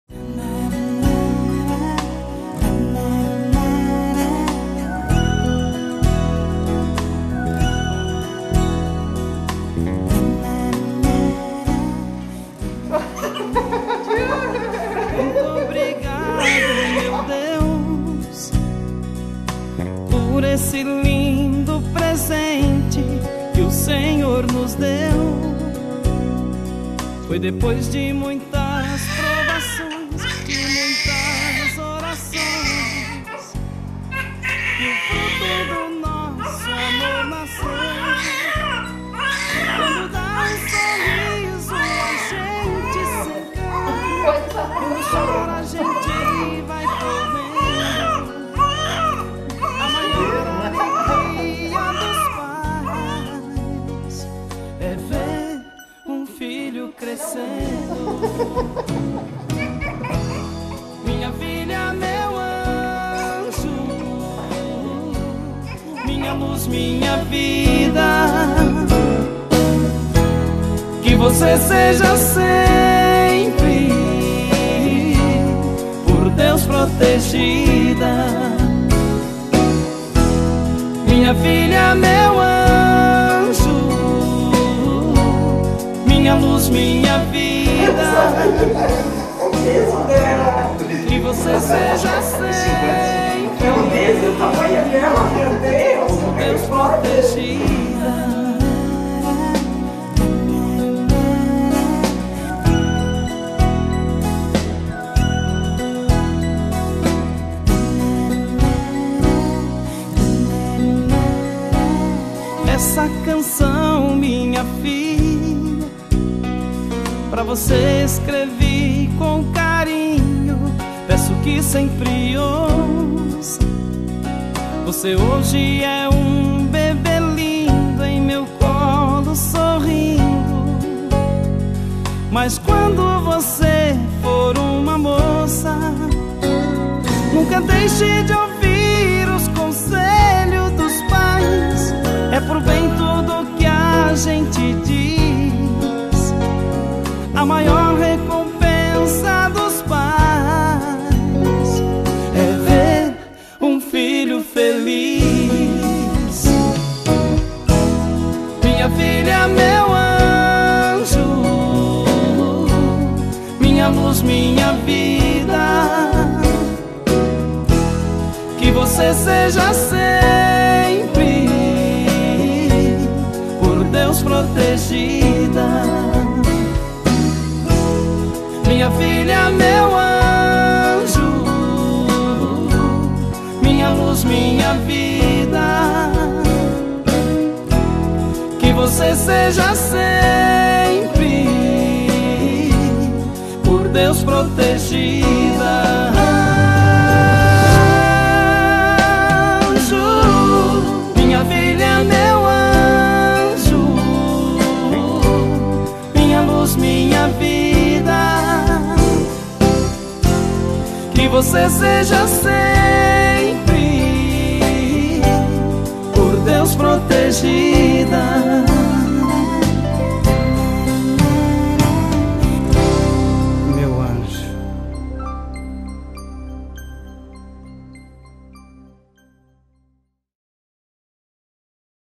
NASCIMENTO EMOCIONANTE
Existem momentos que duram minutos, mas deixam lembranças para toda vida, assim como o nascimento da Beatriz Fiorentin Jacóbus ❤️.